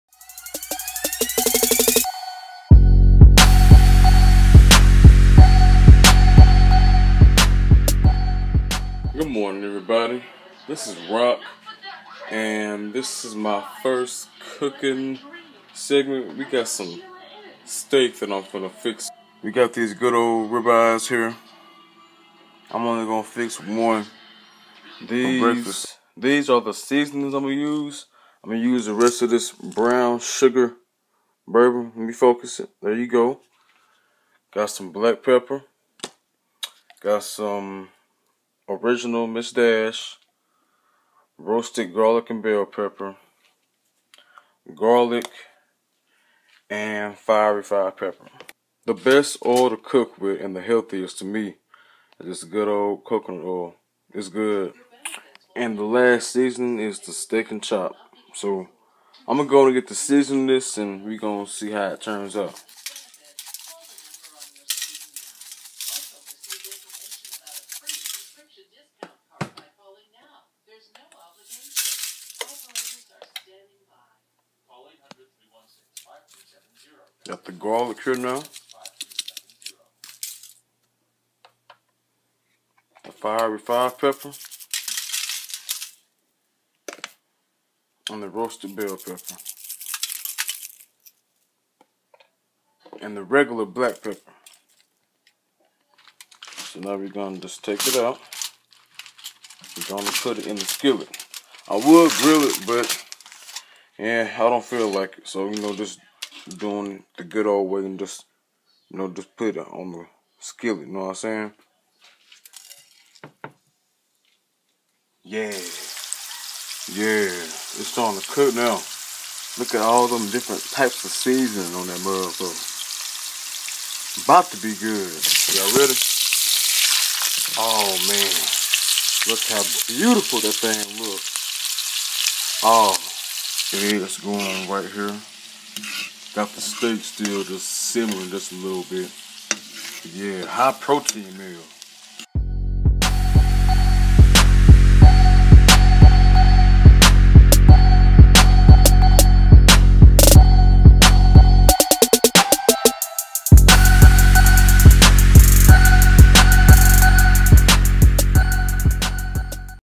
Cooking with Rockk Episode #1
Ribeye Steak and some Eggs. High Protein Powerlifting/Bodybuilding meal
J